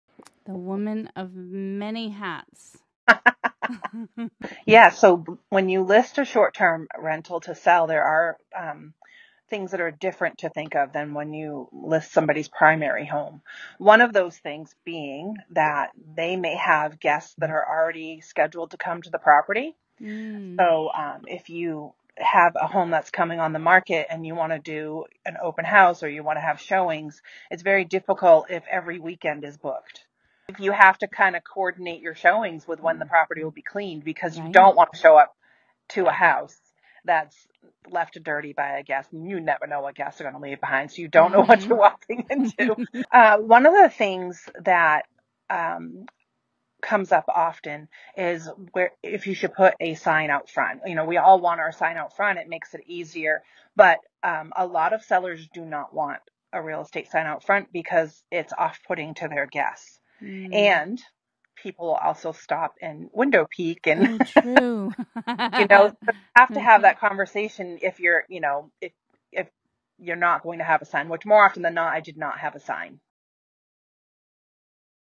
Short-Term Listing Side
Did you miss the second episode featuring Stephanie Wildoner?! Since Stephanie and Julie focused on the buy side on the first podcast episode together, they covered the list side this time! Just as before, LOTS of nuggets of gold within this episode.📝🖊 Tune in for stories from buyers backing out during the walk through😬, to a guest ripping the shower head out of a short-term listing a week before closing!😭🍿